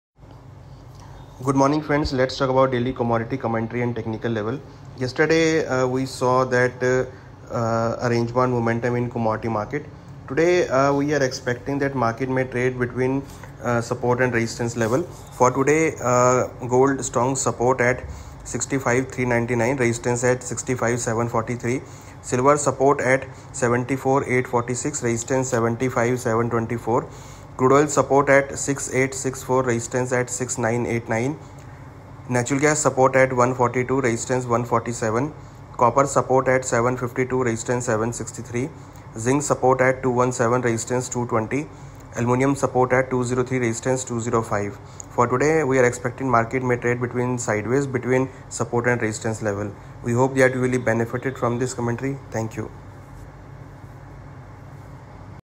Daily Commodity Market Outlook 20.03.2024 by Mr. Anuj Gupta, HDFC Securities
Daily Commodity Market Outlook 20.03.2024  Mr. Anuj Gupta,  HDFC Securities

SEBI Registration number is INZ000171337